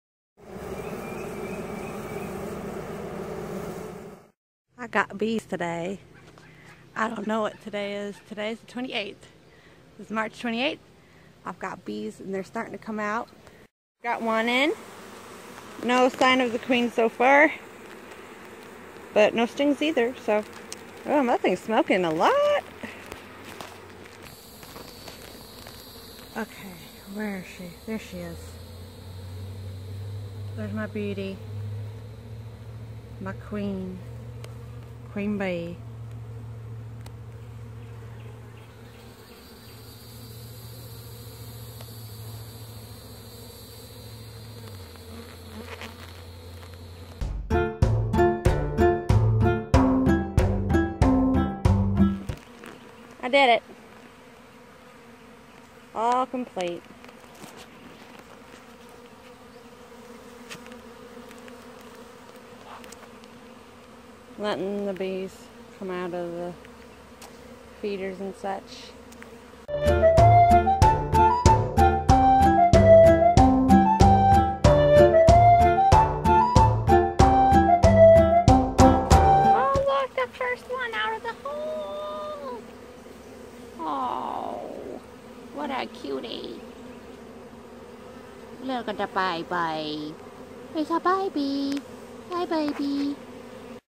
Homestead Vlog [1]
[MAY 28th] The day I became a bee mom
Bout time I start uploading all the bull I've been recording